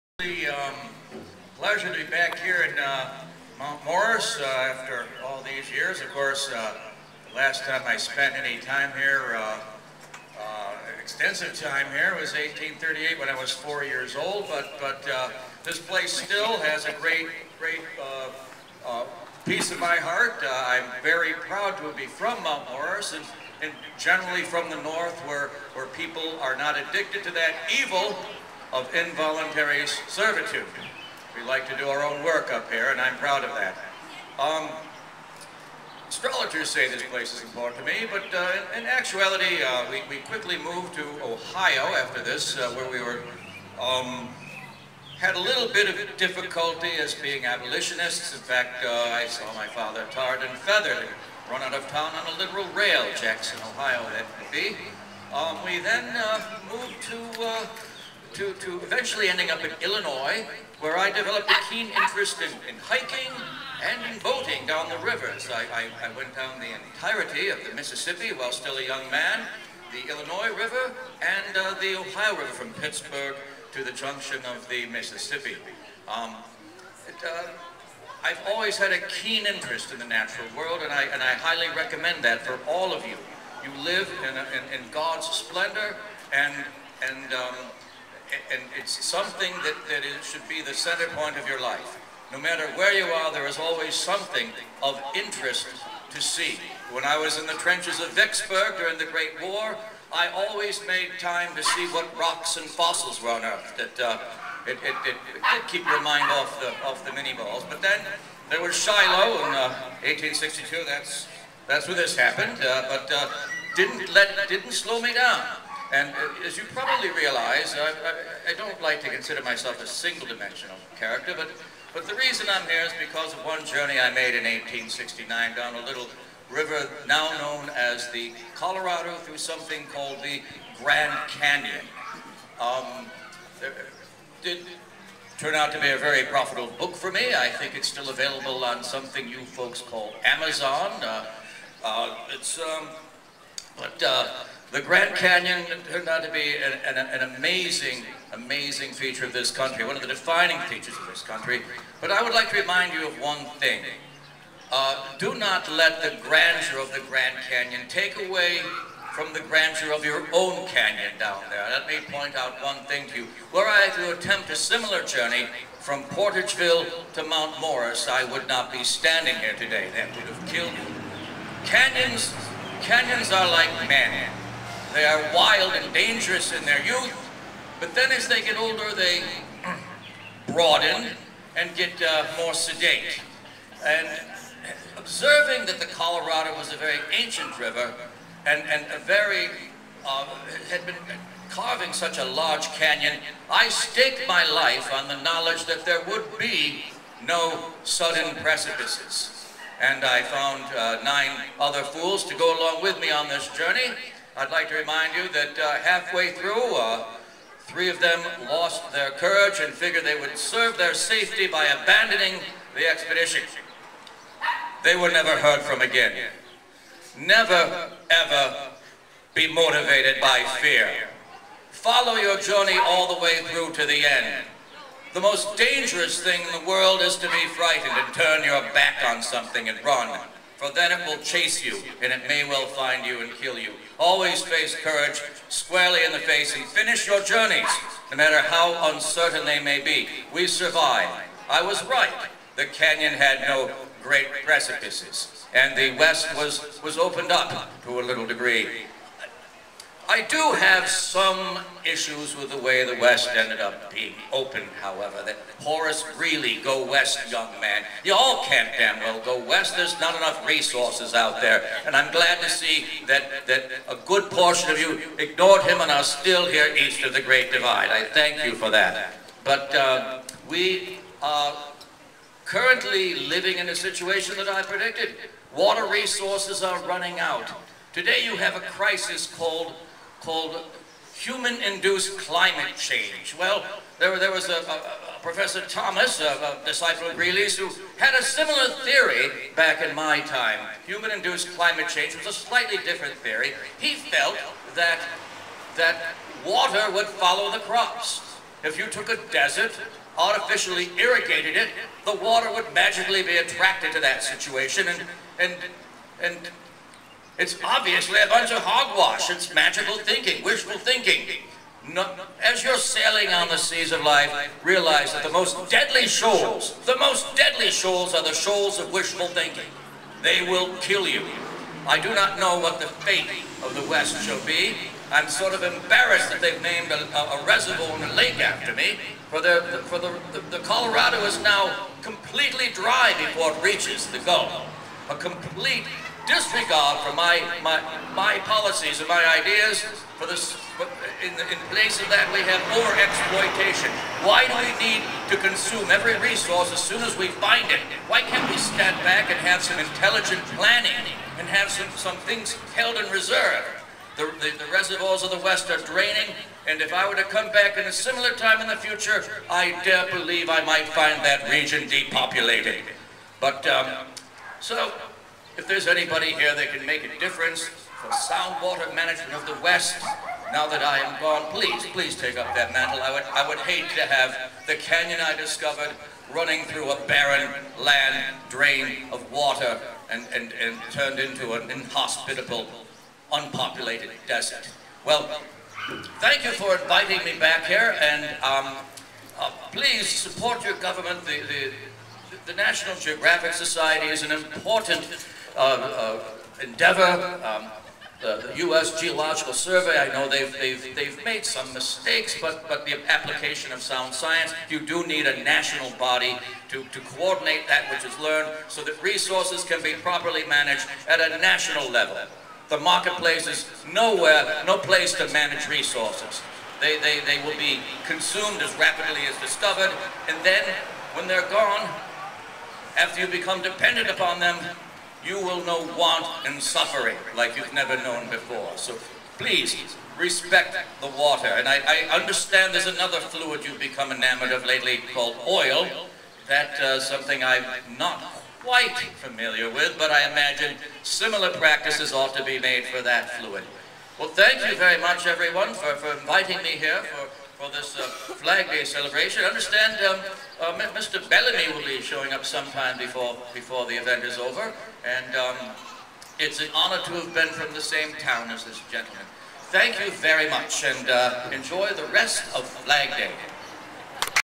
John Wesley Powell, 1800s geologist, as portrayed by Charles Appell
John Wesley Powell, 1800s geologist, as portrayed by Charles Appell at the Mount Morris, NY, Flag Day 2014 celebration.

John Wesley Powell (March 24, 1834 -- September 23, 1902) was a U.S. soldier, geologist, explorer of the American West, professor at Illinois Wesleyan University, and director of major scientific and cultural institutions. He is famous for the 1869 Powell Geographic Expedition, a three-month river trip down the Green and Colorado rivers, including the first known passage through the Grand Canyon. June 14, 2014.